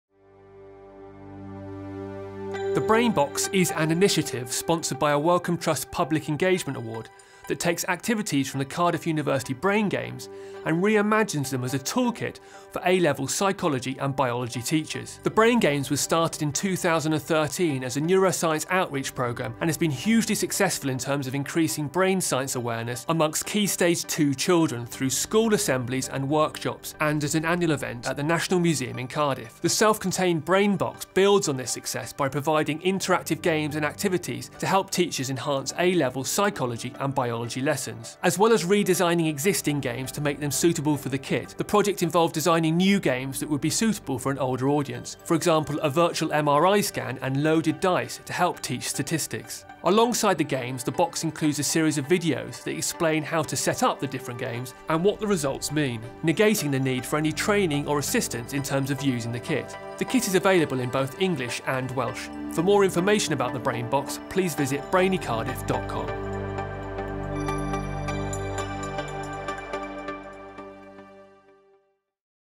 Cardiff University Brain Box
An introduction to the Brain Box from Cardiff University, a kit designed to help teach neuroscience in A-level Psychology and Biology classes. This project was funded by a Wellcome Trust Public Engagement Award.